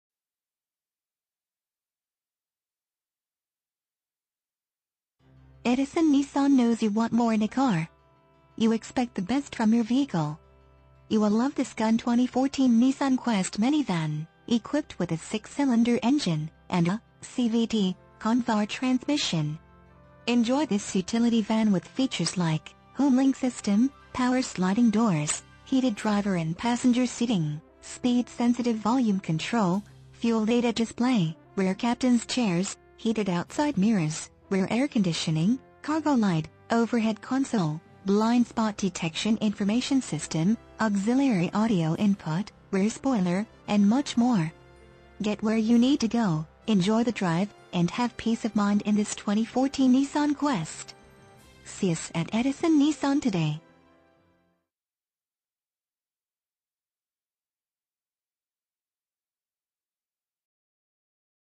2014 Nissan Quest Edison Nissan Edison, NJ 08817
Nissan Quest Edison Nissan knows you want more in a car. You have a purpose for your vehicle. Check out this Gun Nissan Quest Mini Van, equipped with a 6 Cylinder engine and a (cvt) cont var. transmission. Enjoy this utility van with features like Sirius Xm Satellite Radio, Power Liftgate With Closure Assist, Remote Operation Power Windows, Xenon Headlights, Blind Spot Detection Information System, Rear Air Conditioning With Independent Controls, Ambient Lighting, Homelink Universal Garage Door Opener, Auto-Dimming Inside Rearview Mirror, Electronic Stability Control, Power Adjustable Lumbar Driver Seat, Remote Anti-Theft Alarm System, Push-Button Start, Navigation System With Touch Screen Display, Heated Exterior Mirrors With Integrated Turn Signals, Memory Settings For Exterior Mirrors, Electronic Brakeforce Distribution, Power Door Locks With Anti-Lockout Feature, Speed Sensitive Front Wipers, Front Fog Lights, and much more. Get where you need to go, enjoy the drive, and have peace of mind in this Nissan Quest. See us at Edison Nissan today!
MILES:  1,858
VIN:JN8AE2KP1E9102877

Edison Nissan
401 Route 1 South
Edison, New Jersey 08817